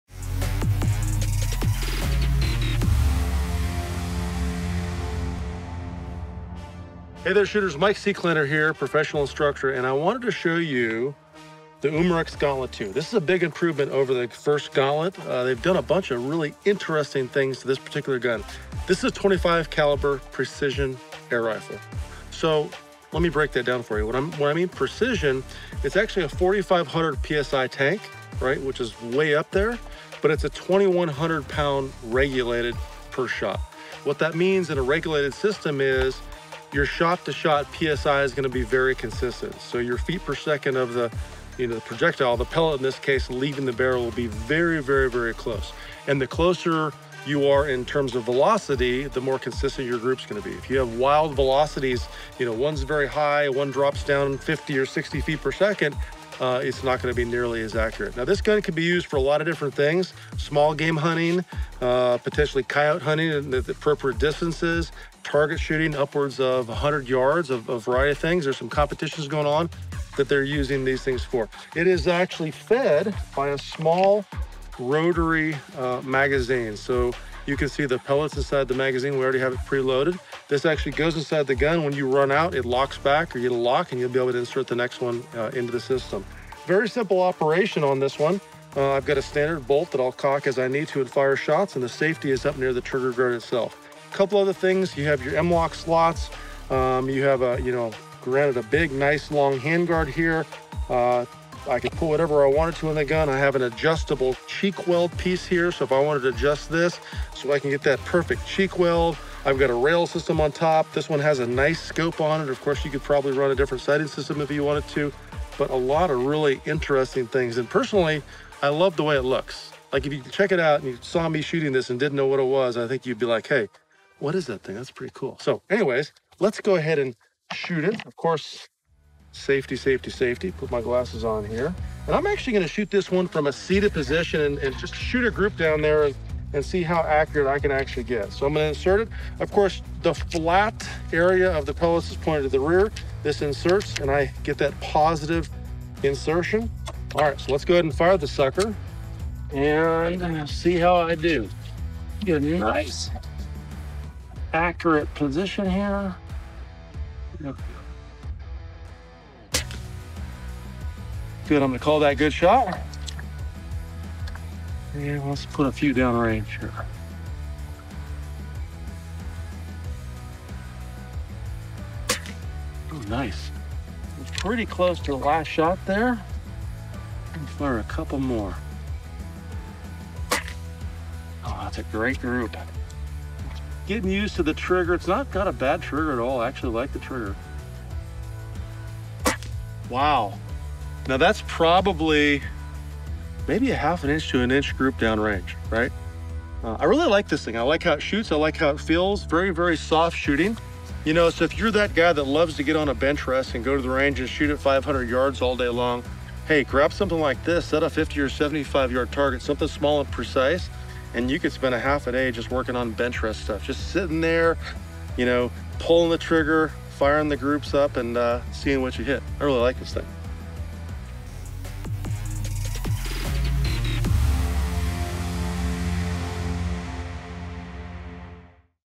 Hunt With This Air Rifle
Ready to run the Gauntlet? Umarex's new precision .25 caliber air gun will impresses small game and predator hunters where legal. Watch the video and share your thoughts.